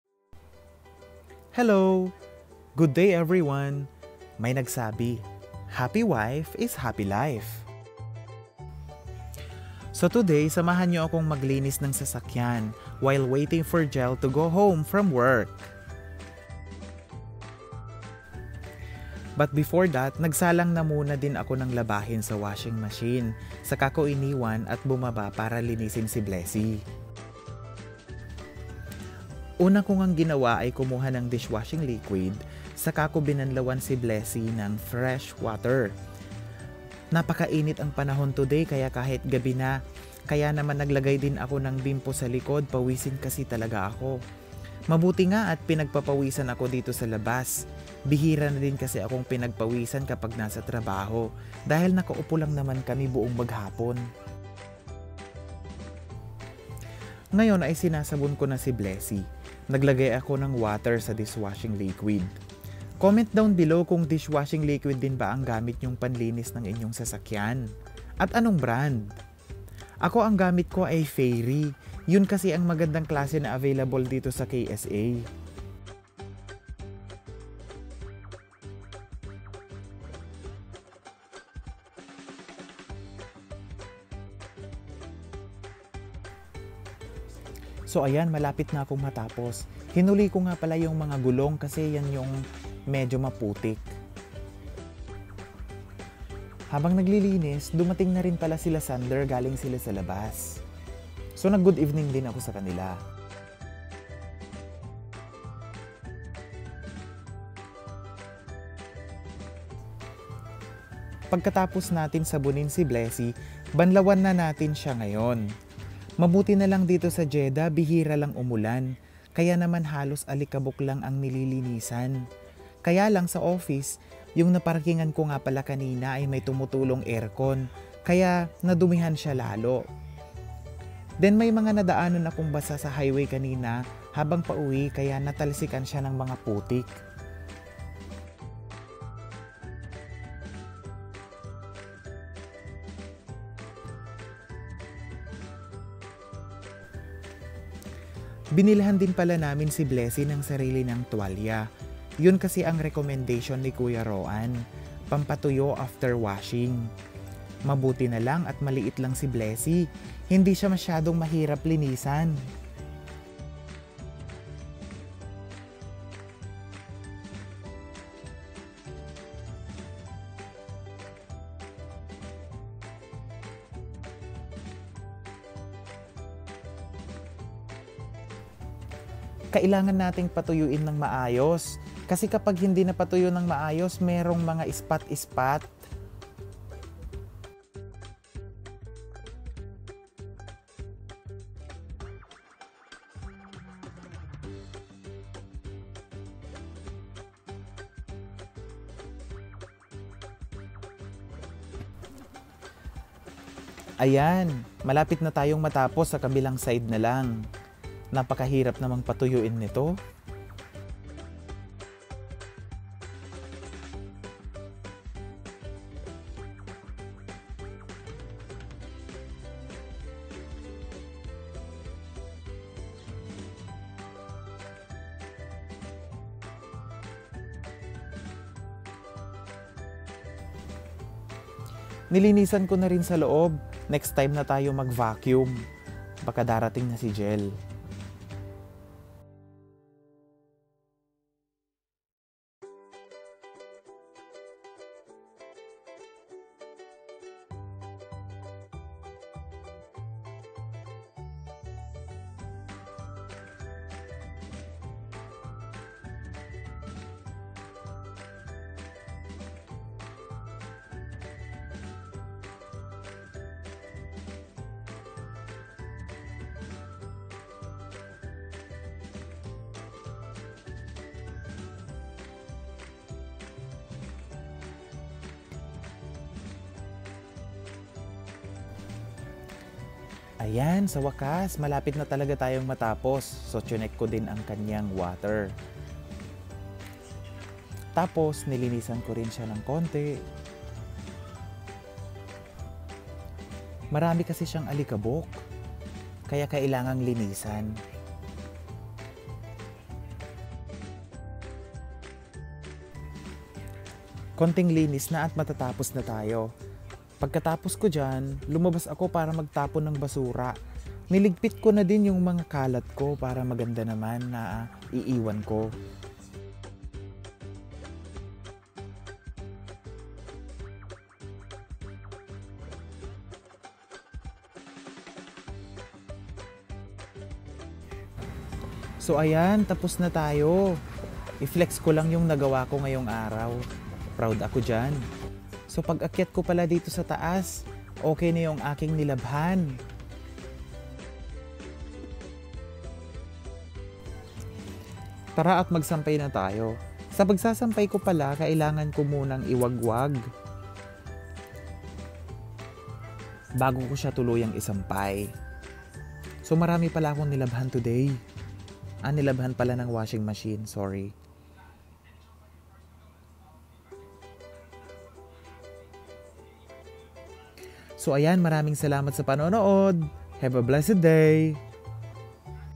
Wednesday is washing day
While waiting my wife to go home from work. I need to do some household chores 😇😃